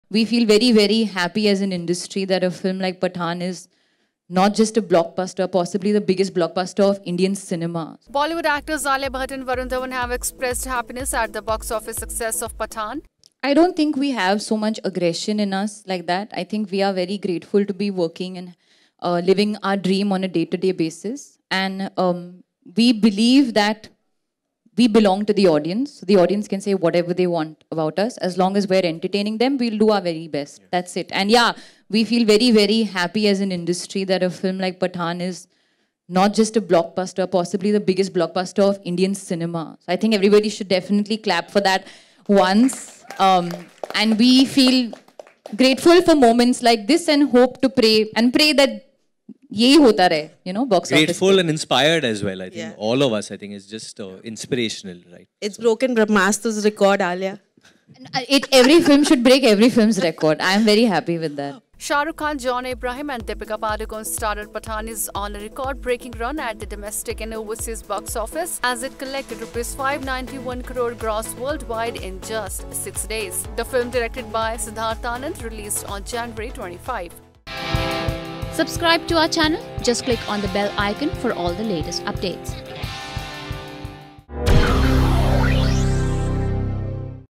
Alia Bhatt and Varun Dhawan react to the success of 'Pathaan'
1.Bollywood actors Alia Bhatt and Varun Dhawan have expressed happiness at the box office success of 'Pathaan'.